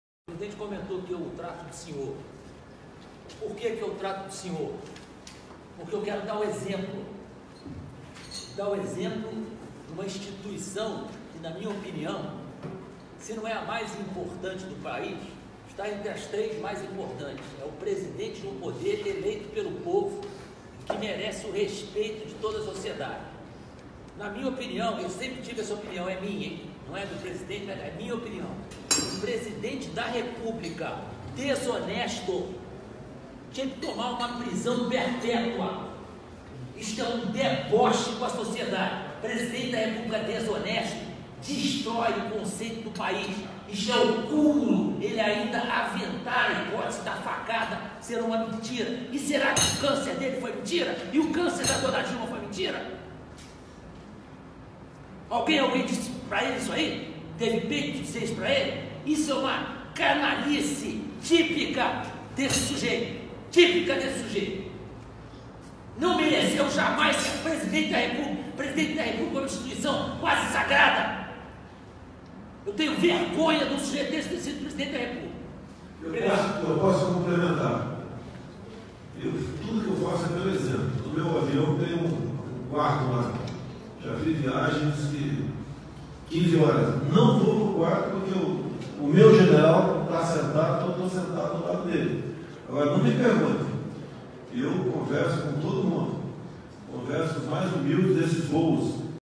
GENERAL HELENO FALA DE CÂNCER DE LULA - Ministro de Bolsonaro detona petista em vídeo
O General Heleno partiu pra cima do ex-presidente Lula. "Ele não mereceu ser presidente da República", afirmou o ministro.